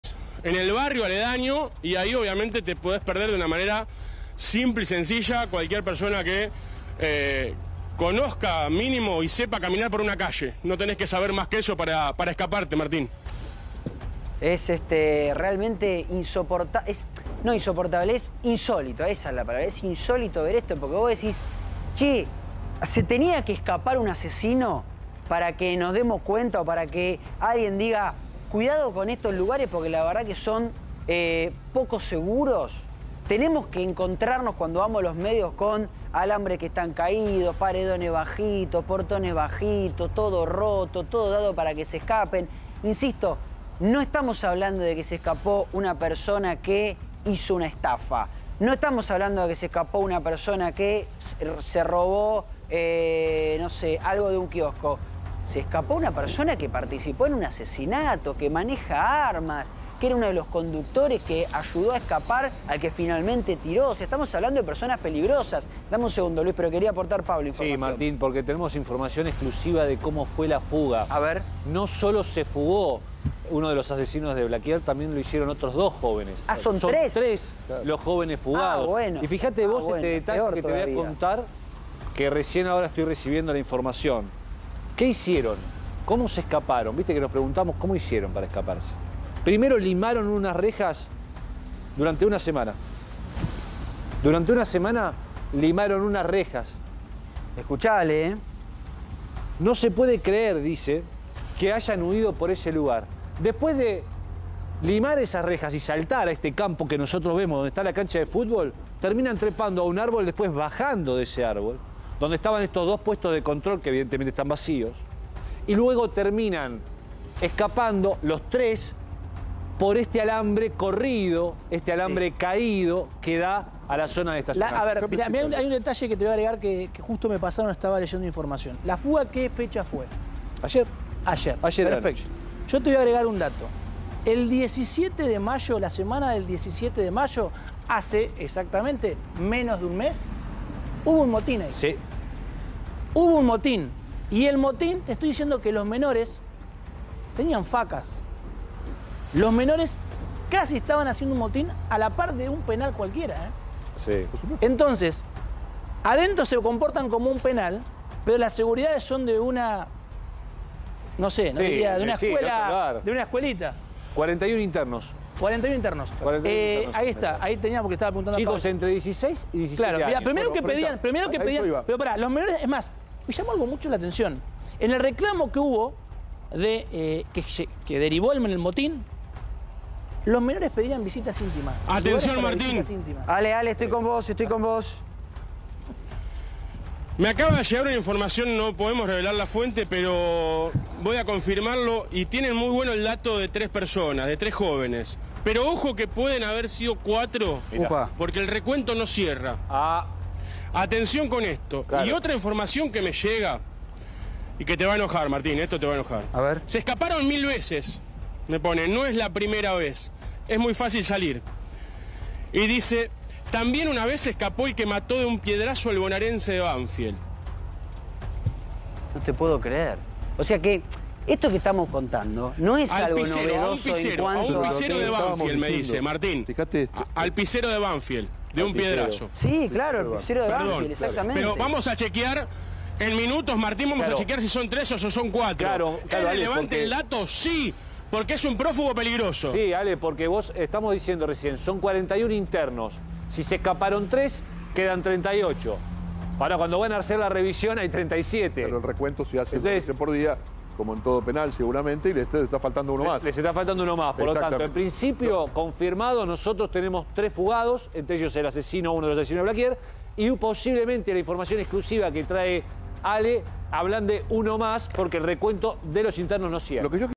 Así funciona un instituto de menores: había habido un motín antes de la fuga del asesino de Blaquier
Antes de la fuga del asesino de Blaquier había habido un motín.